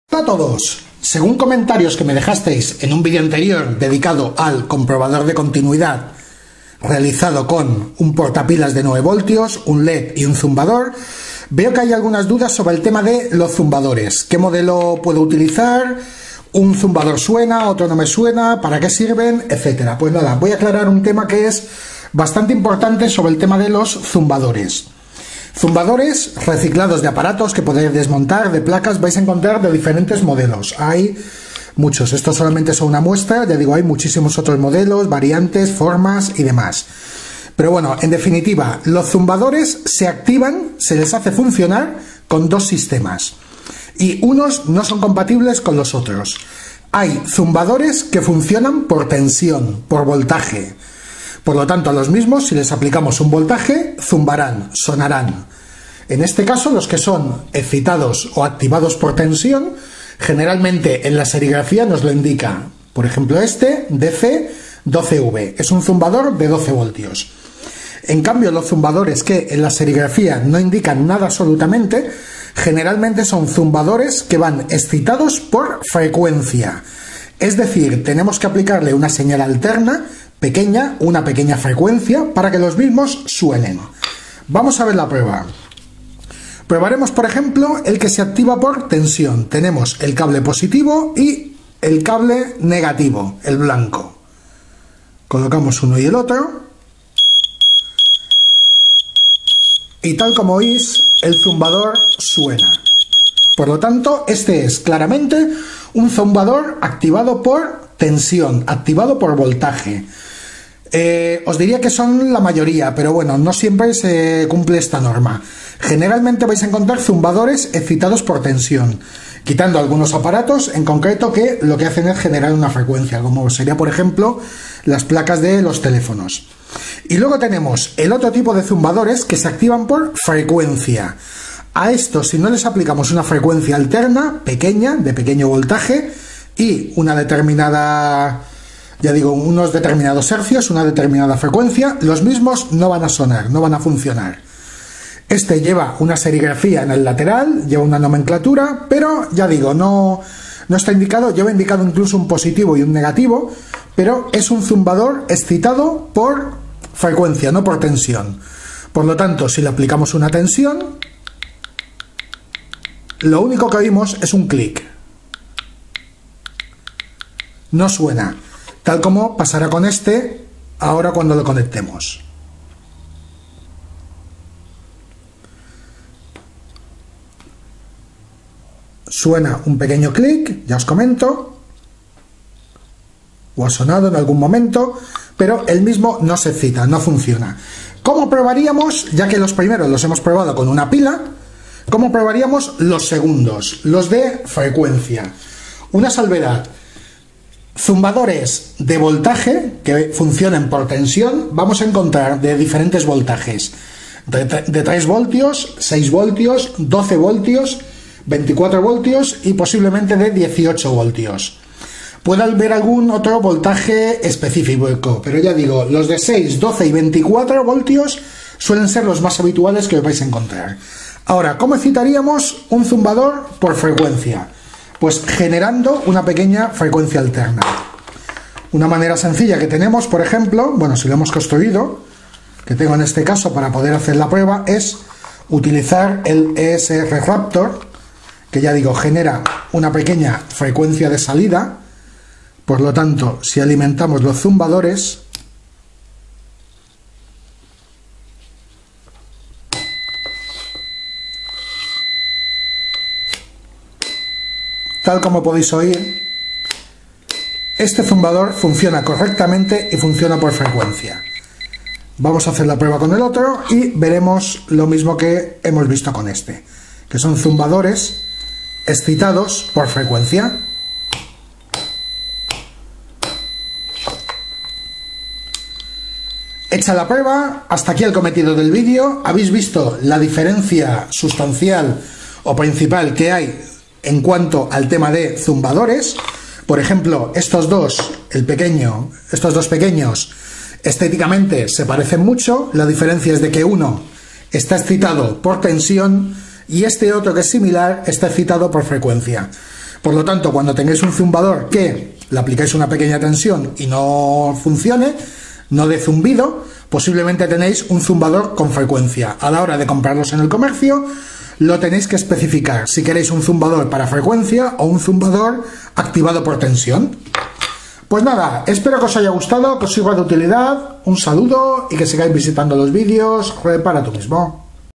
ELECTRONICA BASICA: COMO DIFERENCIAR LOS DOS TIPOS DE FUNCIONAMIENTO DE ZUMBADORES
Vídeo de electrónica Básica, dedicado a conocer los dos tipos de tecnologías de funcionamiento de ZUMBADORES, O CHICHARRAS, que hay en el mercado, las que funcionan excitadas por VOLTAJE y las que funcionan excitadas por frecuencia y como podemos distinguirlas fácilmente.